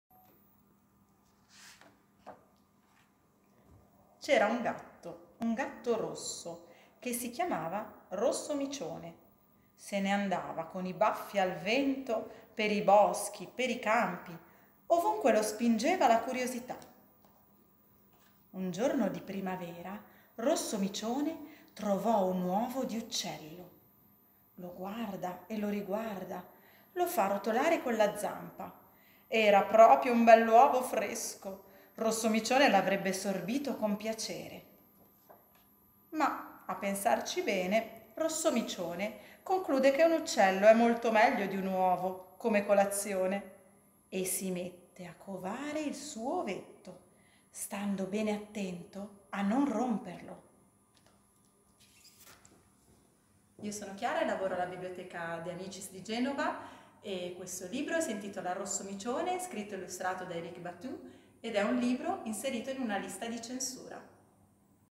Libri salvati - video 5
Rassegna di letture pubbliche di libri proibiti il 10 maggio promossa dall’Associazione italiana biblioteche, in collaborazione con l’Associazione italiana di public history e con il patrocinio dell’Associazione italiana editori